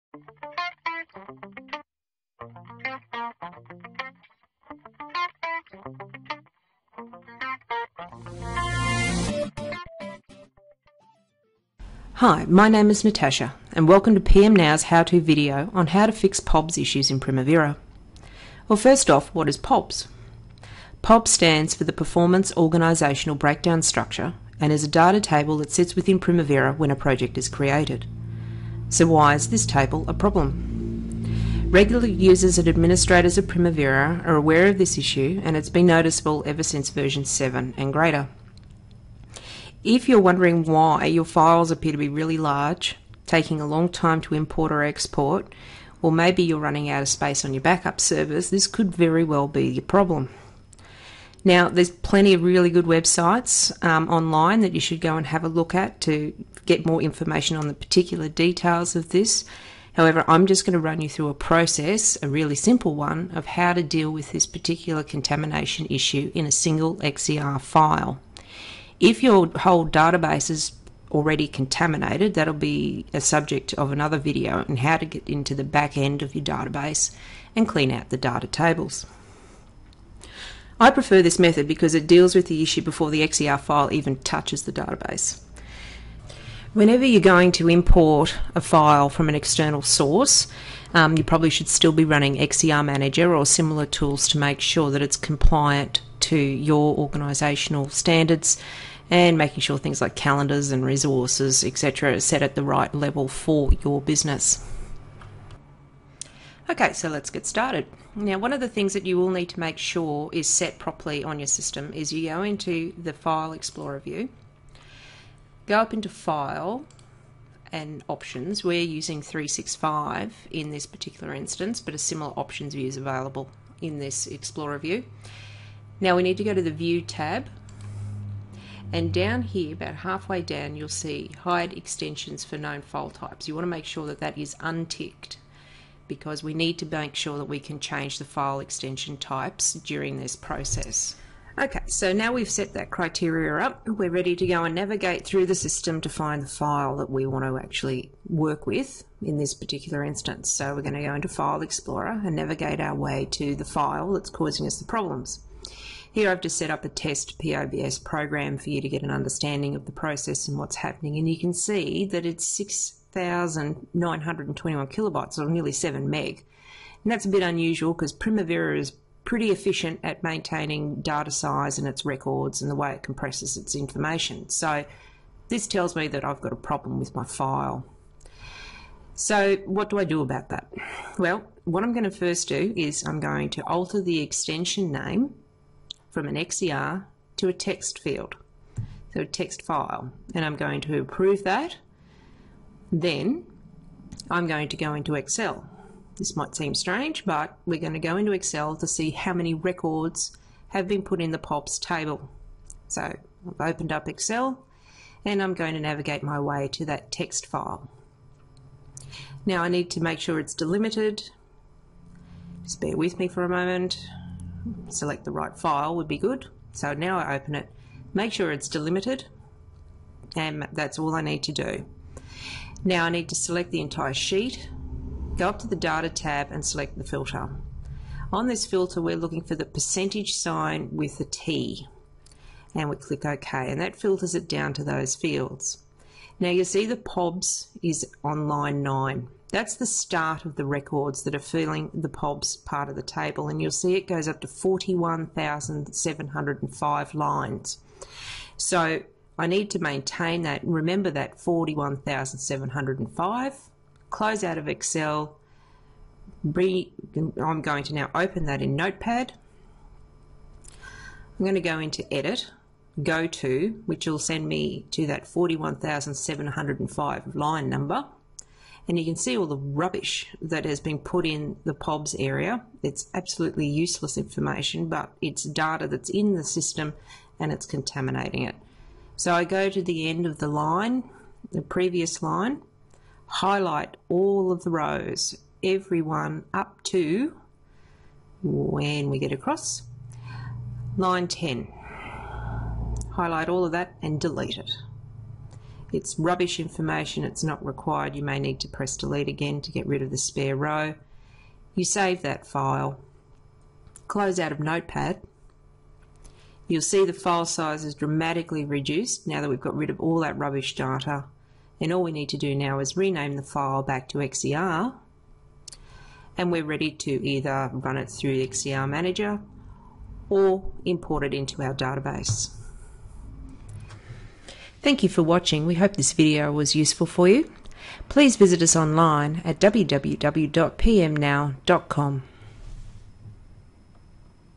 Primavera P6 - How to fix a POBS Issue in a XER File
Primavera P6 - Learn how to reduce your XER file size for your Project Schedules.

Clearing out the POBS Issue prior to importing the file into your Database will help keep things clean and unpolluted with irrelevant data.

Please visit 4Cite and learn more about Project Controls and our services offered in Australia

The PM-Now Network

Share the Knowledge and be part of your local network of project professionals in Australia